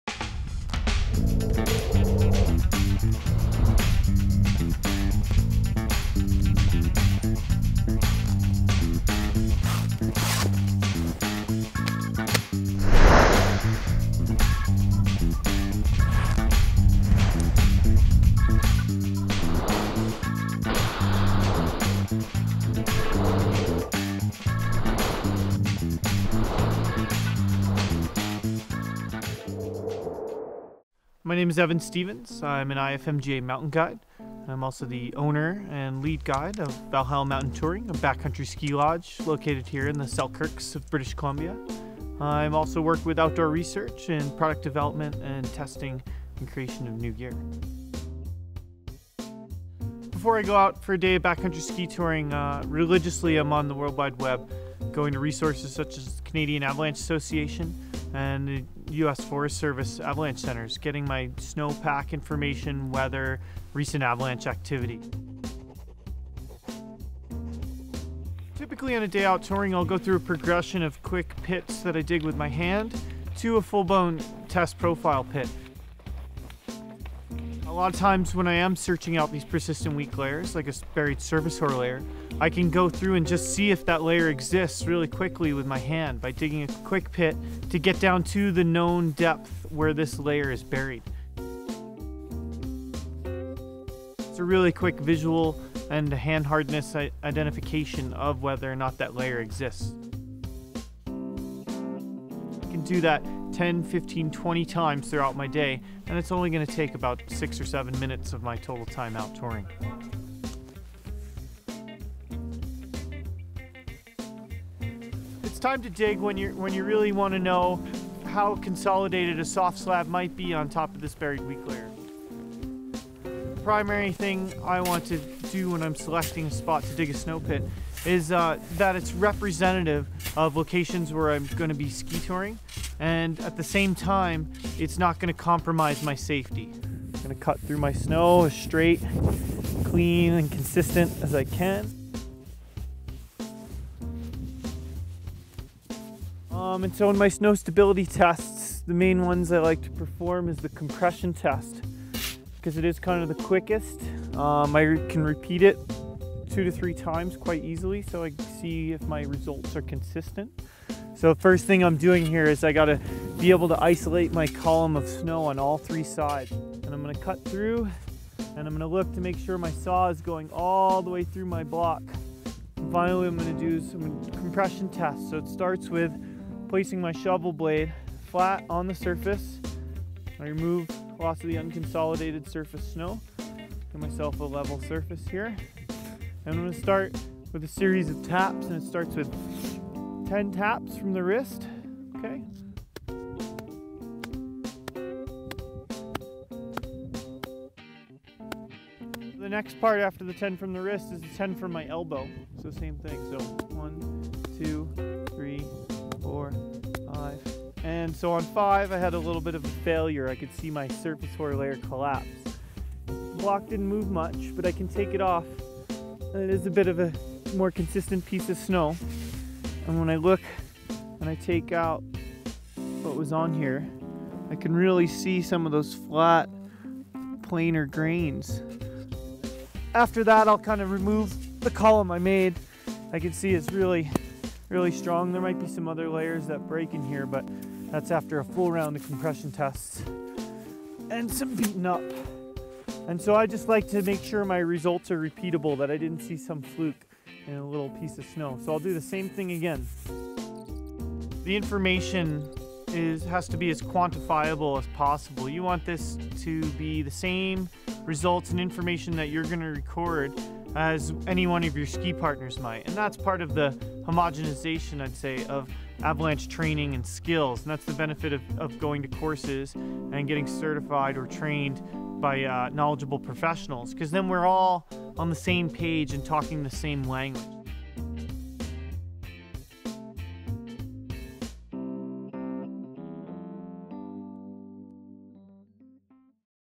Snow Pit Basics w/ IFMGA Guide Evan Stevens
Knowing how to use tools and interpret finding is key to understanding snow pack and making good decisions for safe travel through avalanche terrain. IFMGA guide Evan Stevens takes us through the basics of digging a good snow pit and performing a compression test. Have a great, safe ski season!

As featured on Backcountry Magazine's "Mountain Skills" video series.